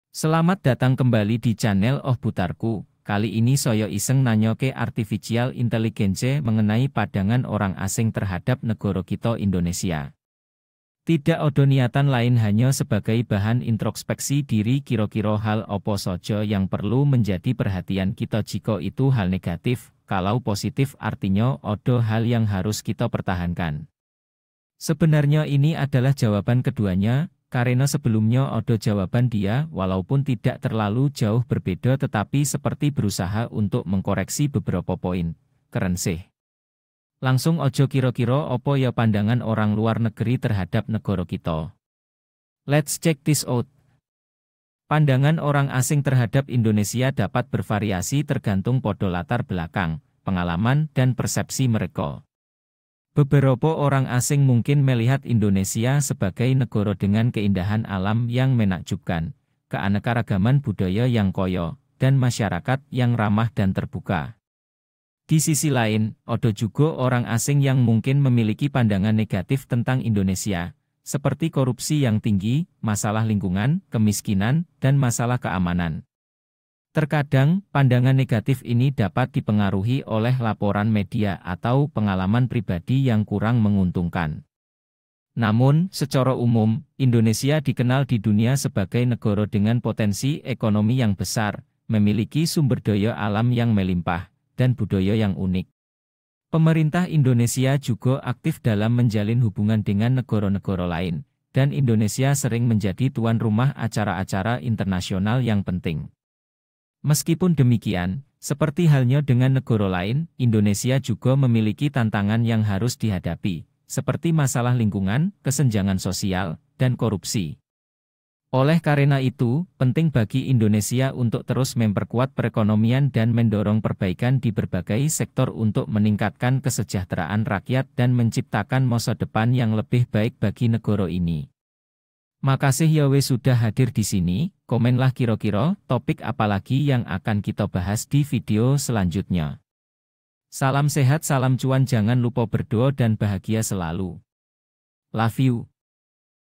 Begini Pandangan Orang Luar Negri Terhadap Negara Indonesia. Ternyata Negara Kita Punya Potensi lhoo
Selamat datang kembali di channel Oh butarku, kali ini saya iseng nanya ke Artificial Inteligence mengenai padangan orang asing terhadap negara kita Indonesia. Tidak ada niatan lain hanya sebagai bahan introkspeksi diri kira-kira hal apa saja yang perlu menjadi perhatian kita jika itu hal negatif, kalau positif artinya ada hal yang harus kita pertahankan.
Sebenarnya ini adalah jawaban keduanya, karena sebelumnya ada jawaban dia walaupun tidak terlalu jauh berbeda tetapi seperti berusaha untuk mengkoreksi beberapa point, keren sih
langsung aja kira-kira apa ya pandangan orang luar negeri terhadap negara kita??? lets check this out :

Pandangan orang asing terhadap Indonesia dapat bervariasi tergantung pada latar belakang, pengalaman, dan persepsi mereka. Beberapa orang asing mungkin melihat Indonesia sebagai negara dengan keindahan alam yang menakjubkan, keanekaragaman budaya yang kaya, dan masyarakat yang ramah dan terbuka.

Di sisi lain, ada juga orang asing yang mungkin memiliki pandangan negatif tentang Indonesia, seperti korupsi yang tinggi, masalah lingkungan, kemiskinan, dan masalah keamanan. Terkadang, pandangan negatif ini dapat dipengaruhi oleh laporan media atau pengalaman pribadi yang kurang menguntungkan.

Namun, secara umum, Indonesia dikenal di dunia sebagai negara dengan potensi ekonomi yang besar, memiliki sumber daya alam yang melimpah, dan budaya yang unik. Pemerintah Indonesia juga aktif dalam menjalin hubungan dengan negara-negara lain, dan Indonesia sering menjadi tuan rumah acara-acara internasional yang penting.

Meskipun demikian, seperti halnya dengan negara lain, Indonesia juga memiliki tantangan yang harus dihadapi, seperti masalah lingkungan, kesenjangan sosial, dan korupsi. Oleh karena itu, penting bagi Indonesia untuk terus memperkuat perekonomian dan mendorong perbaikan di berbagai sektor untuk meningkatkan kesejahteraan rakyat dan menciptakan masa depan yang lebih baik bagi negara ini.

Makasih ya we sudah hadir disini, komenlah kira-kira topik apalagi yang akan kita bahas di video selanjutnya.
salam sehat salam cuan jangan lupa berdoa dan bahagia selalu.
lafyu